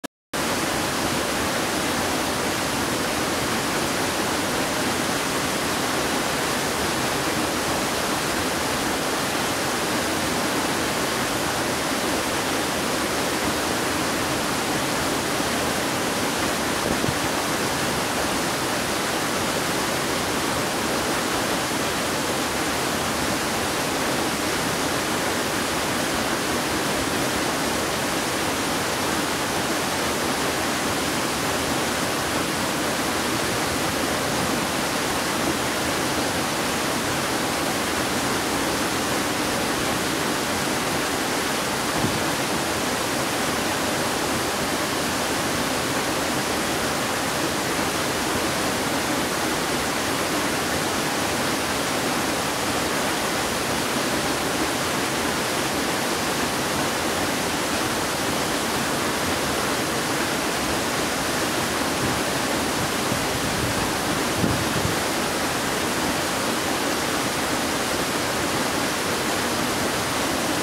VR360 - Chute du camp, between the two waterfalls
Virtual Reality video of a waterfall near Quebec city.